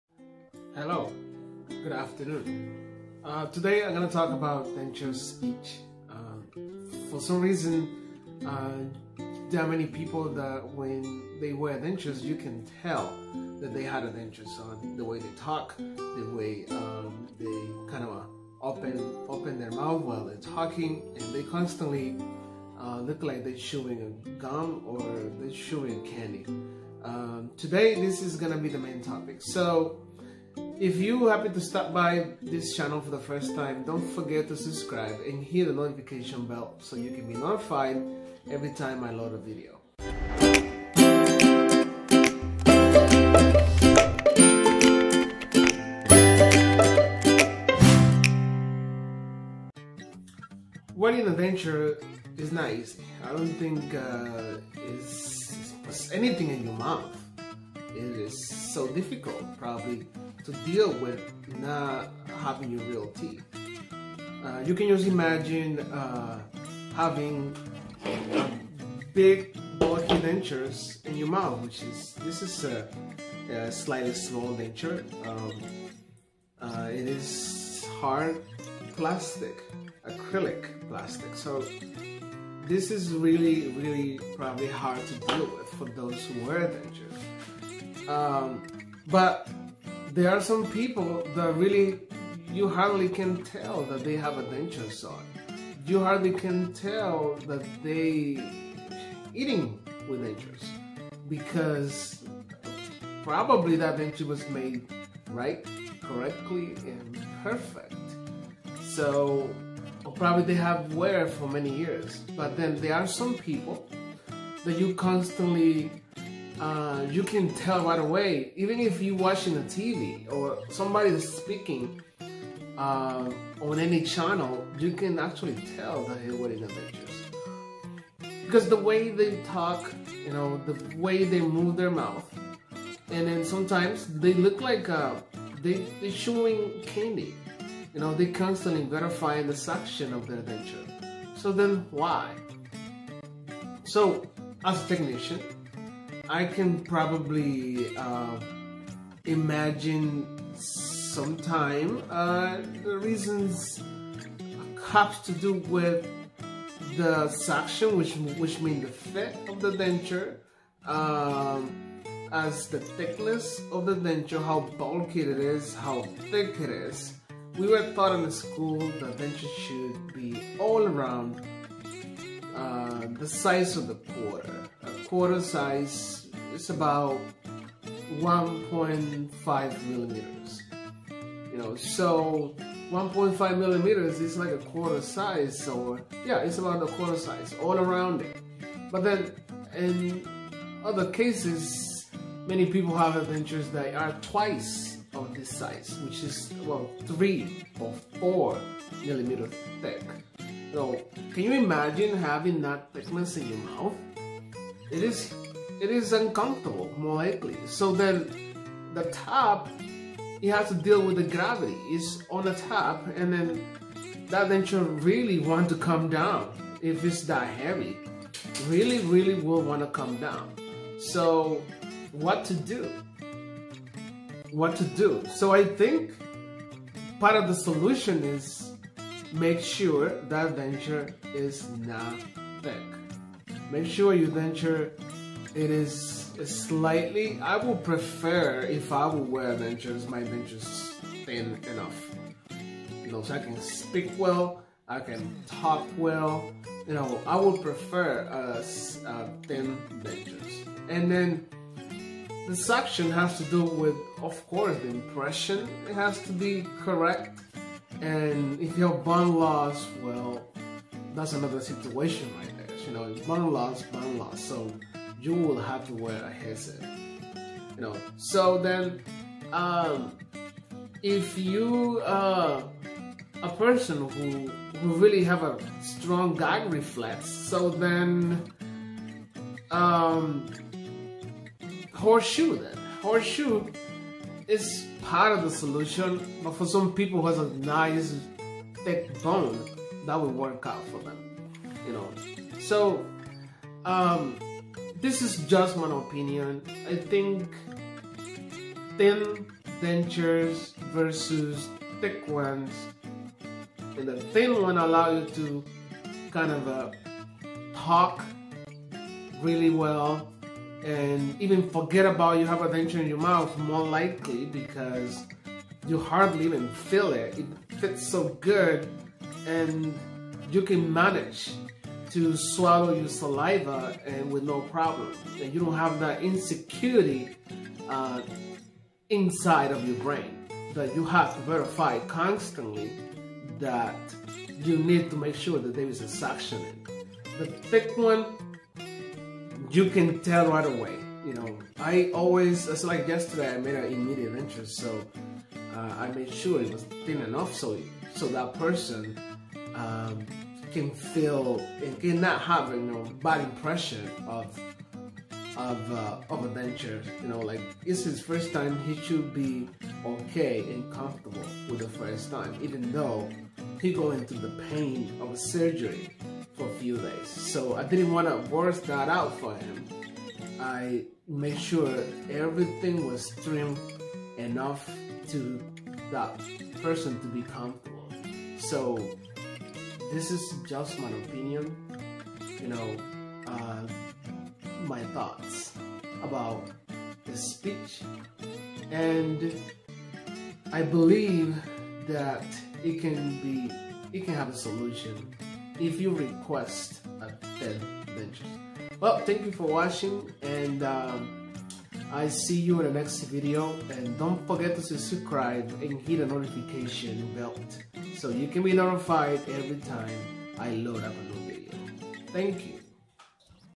HOW THICK DENTURES, AFFECTS YOU SPEECH
This video is about how dentures can affect your speech, and how a "thick" denture can make people look like they are chewing a mouthful of candy, while constantly "sucking their mouth in" to make sure the denture stays in place. Based on my experience, I will offer some solutions in the video while talking about the difference between dentures that are thick or thin. in dentistry.